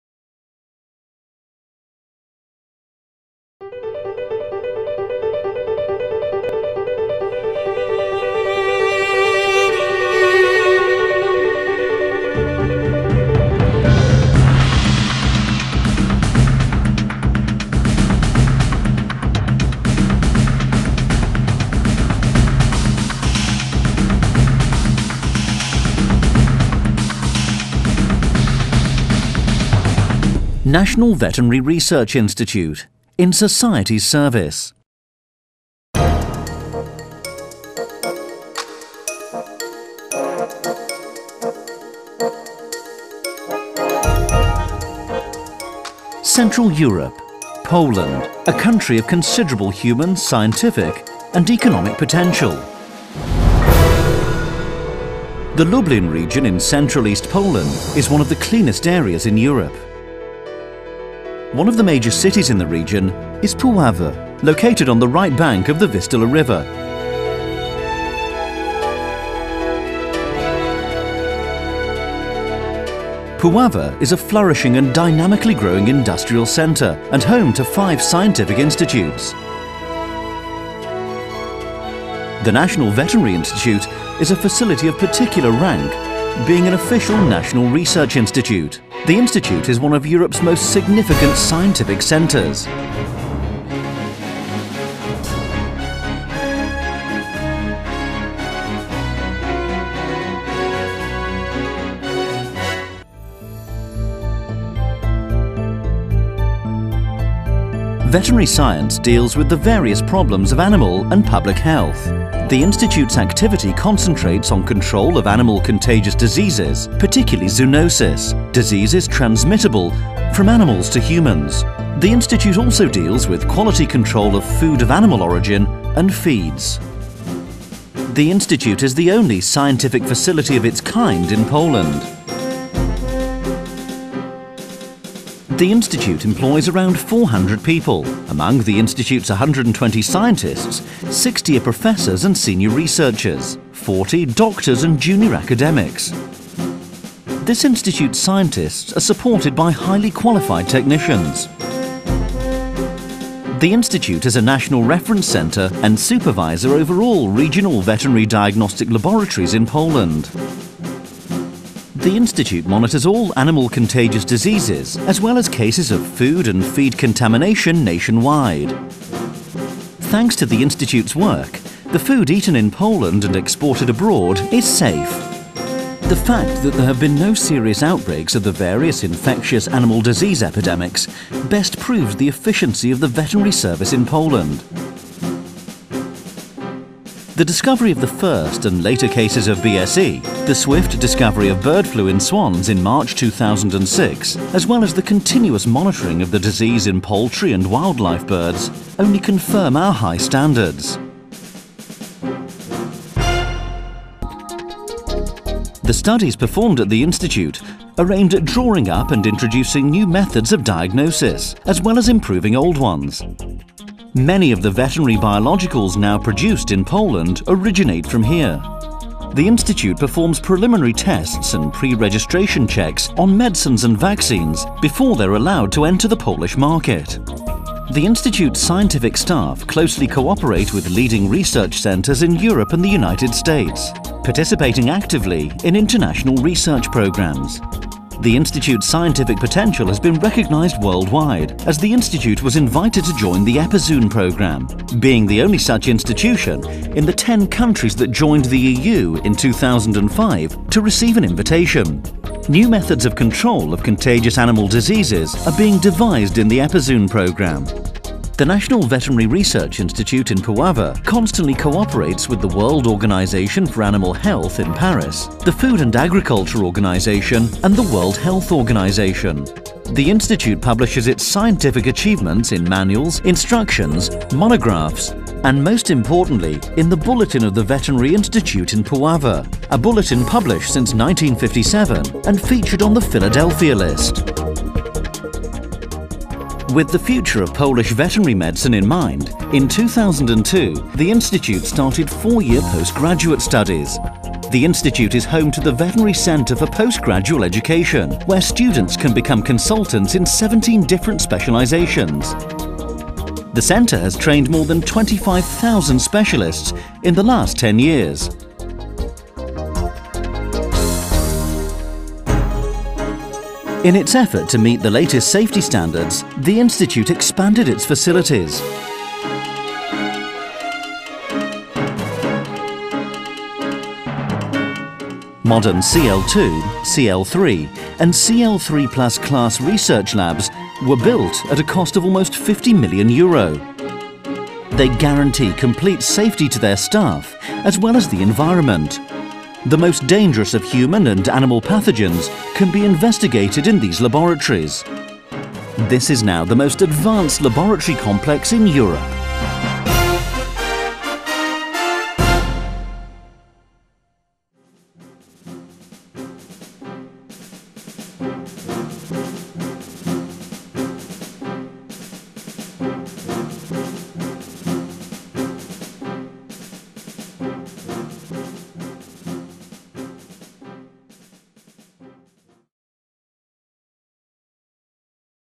PIWet Państwowy Instytut Weterynaryjny w Puławach FILM
PAŃSTWOWY INSTYTUT WETERYNARYJNY został powołany w 1945 r. jako jednostka badawczo-rozwojowa Ministerstwa Rolnictwa i Rozwoju Wsi. W roku 2003 zyskał status państwowego instytutu badawczego i prawo używania nazwy Państwowy Instytut Weterynaryjny - Państwowy Instytut Badawczy w Puławach. Podstawowym zadaniem Instytutu jest prowadzenie badań naukowych w zakresie medycyny weterynaryjnej ze szczególnym uwzględnieniem diagnostyki i profilaktyki chorób zakaźnych, w tym chorób odzwierzęcych, oraz higieny i toksykologii żywności pochodzenia zwierzęcego. Ponadto Instytut prowadzi dokształcanie podyplomowe oraz specjalizacyjne dla lekarzy weterynarii w Weterynaryjnym Centrum Kształcenia Podyplomowego, jest krajowym ośrodkiem referencyjnym w zakresie oceny sytuacji epidemiologicznej w kraju i monitorowania pozostałości ksenobiotyków w żywności pochodzenia zwierzęcego, prowadzi urzędową kontrolę i badania przedrejestracyjne weterynaryjnych preparatów biologicznych, sprawuje opiekę merytoryczną nad zakładami higieny weterynaryjnej oraz pełni rolę doradcy naukowego administracji weterynaryjnej.